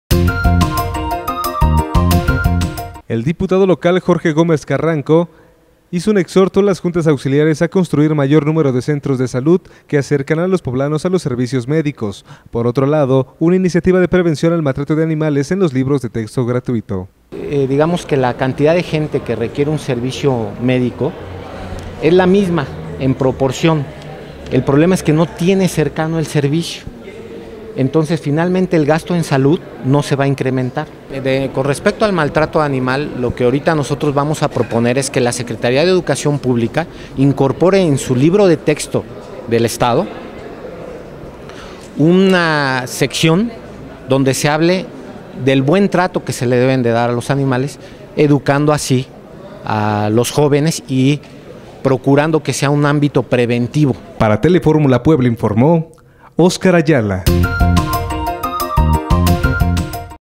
JORGE GOMEZ CARRANCO INICIATIVAS ANTE CONGRESO DEL ESTDO
Incorpore en libro de teto incorpore una sección, ámbito preventivo cuidado a los animales.
Entre algunas de las iniciativas que propone ante sesión pública el Diputado Local Jorge Gómez Carranco es el exhorto a las juntas auxiliares a  construir mayor número de centros de salud que acerquen a los poblanos a los servicios médicos, por otro lado, una iniciativa de prevención al maltrato animal en los libros de texto gratuito.